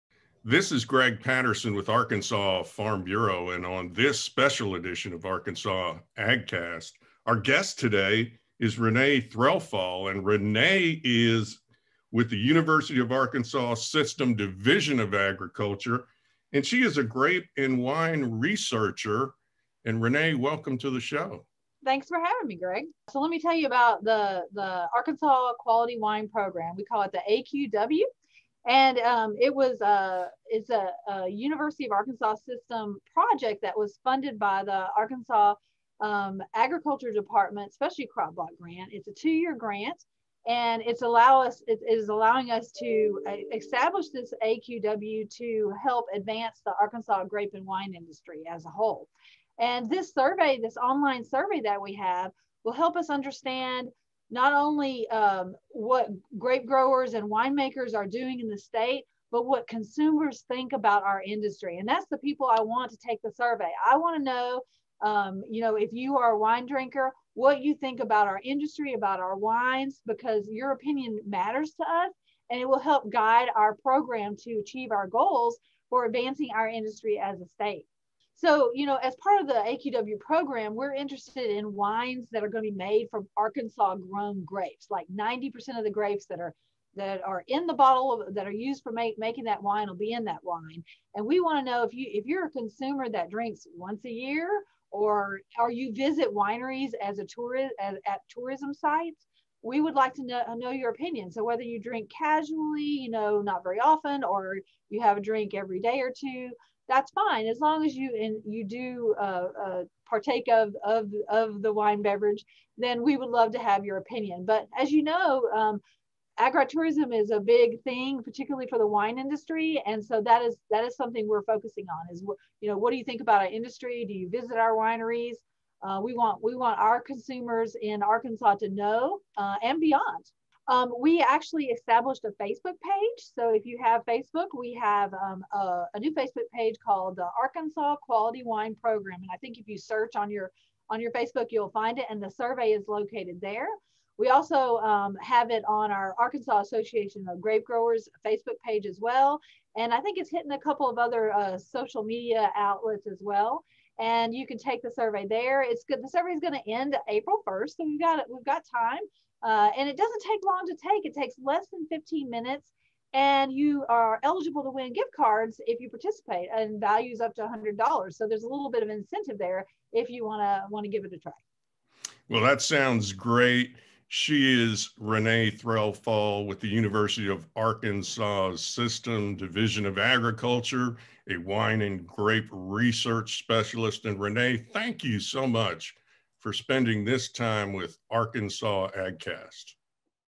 AgCast Video Extra | Renee Threlfall, Grape & Wine Researcher
Renee Threlfall is a grape and wine researcher for the University of Arkansas System Extension - UAEX. She helps lead the Arkansas Quality Wine program. AQW is presently conducting an online consumer survey about Arkansas wines and ag tourism visits to Arkansas wineries. Threlfall talks about the survey which runs online through April 1.